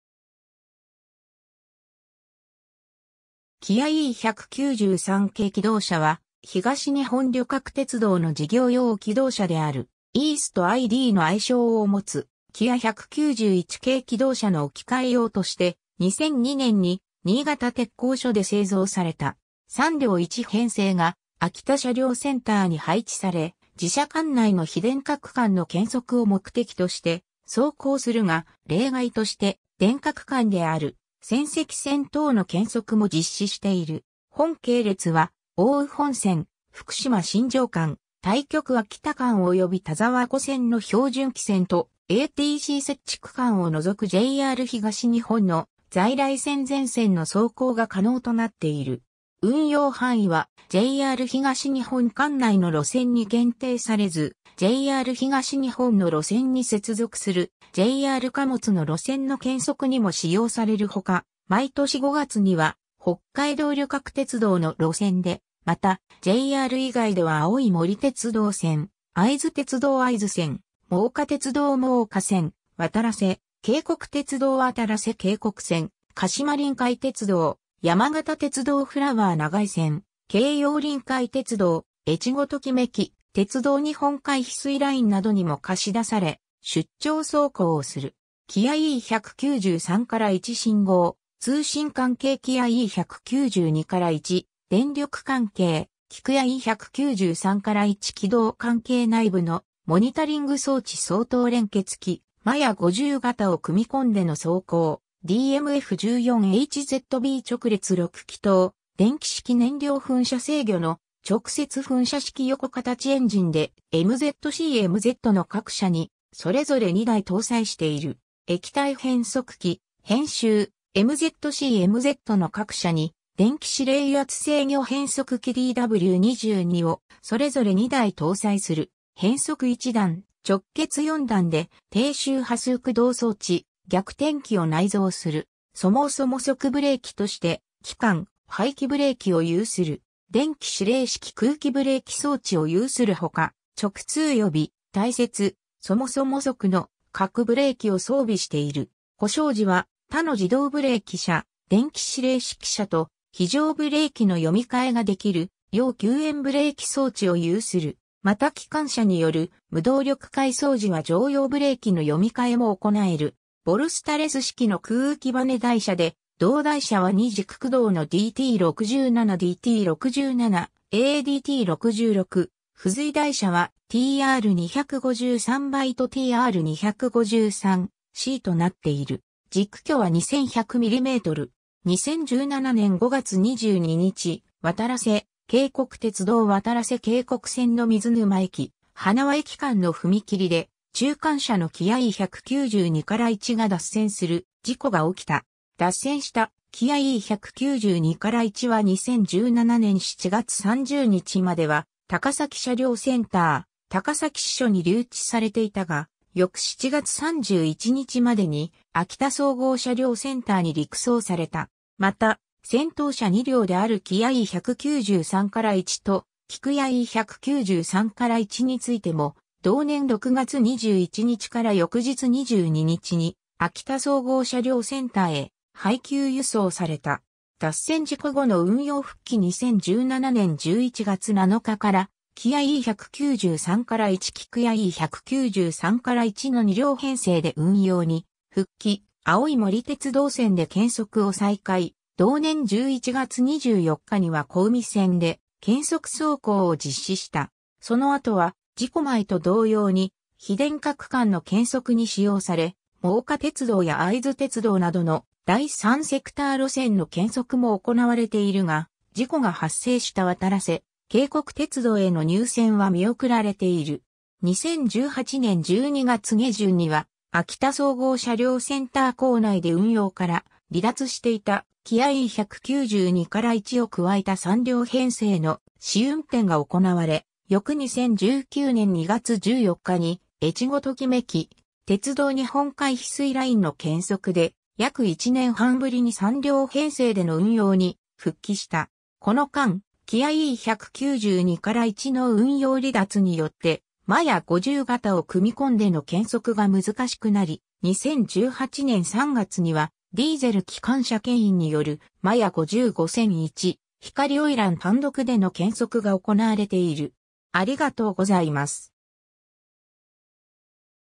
JR東日本キヤE193系気動車
キヤE193系気動車（キヤE193けいきどうしゃ）は、東日本旅客鉄道（JR東日本）の事業用気動車（ディーゼル動車）である。
East i-D（イーストアイ・ダッシュディー）の愛称をもつ。
 キヤ191系気動車の置き換え用として、2002年（平成14年）に新潟鐵工所で製造された。
 3両1編成が秋田車両センターに配置され、自社管内の非電化区間の検測を目的として走行するが、例外として電化区間である仙石線等の検測も実施している。
本系列は、奥羽本線福島 - 新庄間、大曲 - 秋田間および田沢湖線の標準軌線とATC設置区間を除くJR東日本の在来線全線の走行が可能となっている。
 運用範囲はJR東日本管内の路線に限定されず、JR東日本の路線に接続するJR貨物の路線の検測にも使用されるほか、毎年5月には北海道旅客鉄道（JR北海道）の路線で、またJR以外では青い森鉄道線、会津鉄道会津線、真岡鐵道真岡線、わたらせ渓谷鐵道わたらせ渓谷線、鹿島臨海鉄道、山形鉄道フラワー長井線、京葉臨海鉄道、えちごトキめき鉄道日本海ひすいラインなどにも貸し出され、出張走行をする。
 キヤE193-1 (Mzc) 信号・通信関係 キヤE192-1 (Mz) 電力関係 キクヤE193-1 (Tzc) 軌道関係 内部のモニタリング装置 双頭連結器 マヤ50形を組み込んでの走行 DMF14HZB（カミンズ製N14R 連続定格出力450 PS/2,100rpm） 直列6気筒・電気式燃料噴射制御の直接噴射式横形（水平シリンダー形）エンジンで、Mzc・Mzの各車にそれぞれ2台搭載している。
 液体変速機[編集] Mzc・Mzの各車に電気指令油圧制御変速機DW22をそれぞれ2台搭載する。
変速1段、直結4段で、（電源用）定周波数駆動装置・逆転機を内蔵する。
抑速ブレーキとして機関・排気ブレーキを有する。
 電気指令式空気ブレーキ装置を有するほか、直通予備、耐雪、抑速（機関・排気）の各ブレーキを装備している。
 故障時は他の自動ブレーキ車・電気指令式車と非常ブレーキの読替が出来るよう救援ブレーキ装置を有する。
また機関車による無動力回送時は常用ブレーキの読替も行える。
 ボルスタレス式の空気ばね台車で、動台車は2軸駆動のDT67（Mzc車 前側）・DT67A（Mzc車 後側）・DT66（Mz車）、付随台車はTR253B（Tzc車 前側）・TR253C（Tzc車 後側）となっている。
軸距は2,100 mm。
 2017年（平成29年）5月22日、わたらせ渓谷鐵道わたらせ渓谷線の水沼駅 - 花輪駅間の踏切で、中間車のキヤE192-1が脱線する事故が起きた。
脱線したキヤE192-1は2017年（平成29年）7月30日までは高崎車両センター高崎支所に留置されていたが、翌7月31日までに秋田総合車両センターに陸送された。
また、先頭車2両であるキヤE193-1とキクヤE193-1についても同年6月21日から翌日22日に秋田総合車両センターへ配給輸送された。
 脱線事故後の運用復帰 2017年11月7日からキヤE193-1+キクヤE193-1の2両編成で運用に復帰、青い森鉄道線で検測を再開、同年11月24日には小海線で検測走行を実施した。
その後は事故前と同様に非電化区間の検測に使用され、真岡鐵道や会津鉄道などの第3セクター路線の検測も行われているが、事故が発生したわたらせ渓谷鐵道への入線は見送られている。
 2018年（平成30年）12月下旬には秋田総合車両センター構内で運用から離脱していたキヤE192-1を加えた3両編成の試運転が行われ、翌2019年2月14日にえちごトキめき鉄道日本海ひすいラインの検測で約1年半ぶりに3両編成での運用に復帰した。
 この間、キヤE192-1の運用離脱によってマヤ50形を組み込んでの検測が難しくなり、2018年3月にはディーゼル機関車牽引によるマヤ50 5001「光オイラン」単独での検測が行われている。
（国鉄50系客車#JR東日本の「建築限界測定車」を参照）